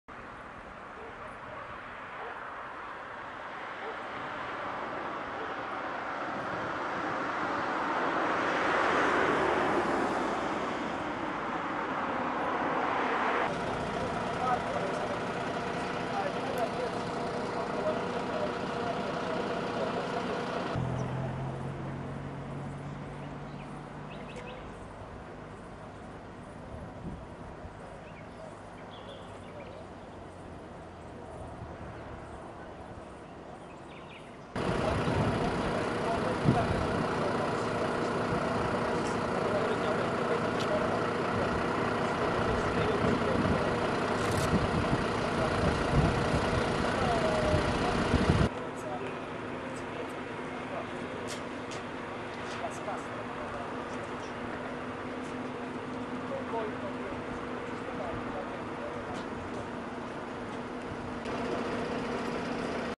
Potenza, autocisterna si incaglia alla svincolo: traffico in tilt per ore
(Agenzia VISTA) - Potenza, 5 Luglio 2016 - Il mancato rispetto del divieto di transito da parte di un mezzo pesante ha bloccato il traffico per diverse ore e fino alle 10 di stamane sul raccordo autostradale Sicignano - Potenza. Protagonista una cisterna per il trasporto di gpl, incagliata nei blocchi in cemento che delimitano il restringimento di carreggiata allo svincolo di Vietri di Potenza - Balvano in direzione di Salerno / TRM